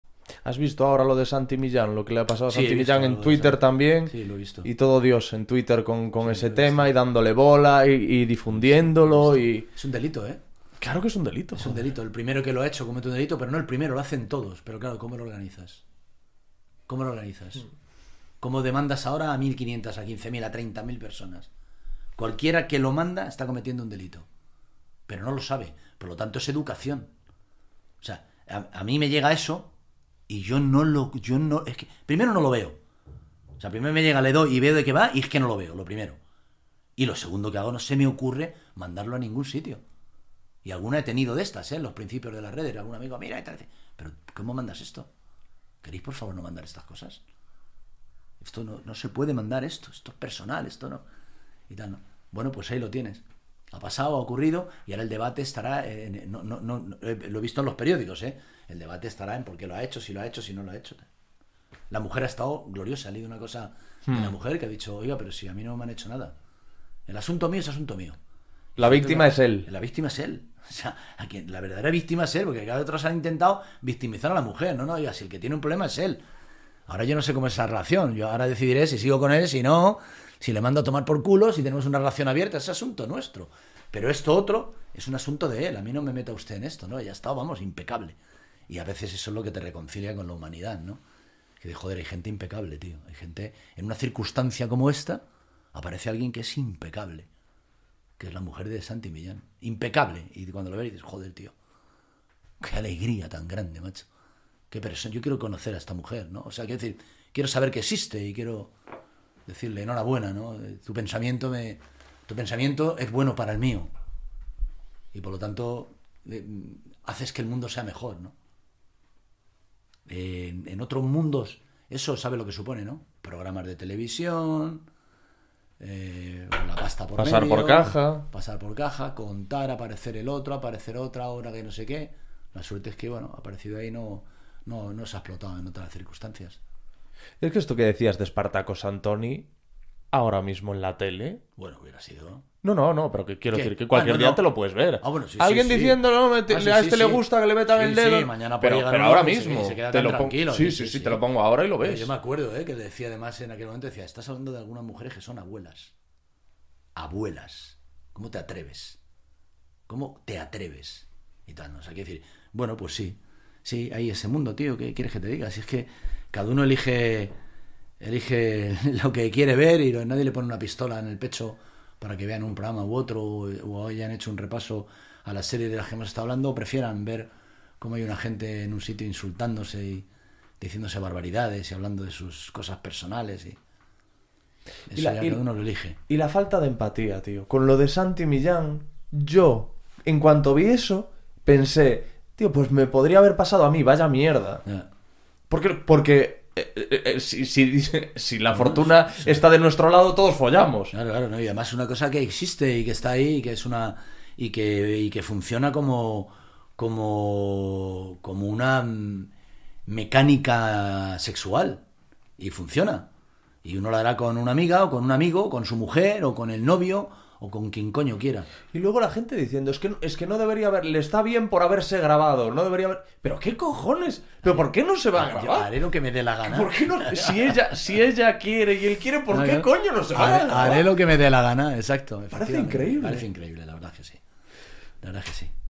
Las Palabras De Ramoncín Sobre La Mujer De Santi Millán
La conversación completa con el rockero, actor y presentador Ramoncín, en el episodiooo 249 del podcast

(asegúrate de hacer clic en la campana para no perderte nuevos videos)

MI LIBRO

•"Lo que tú digas" (editorial Kailas)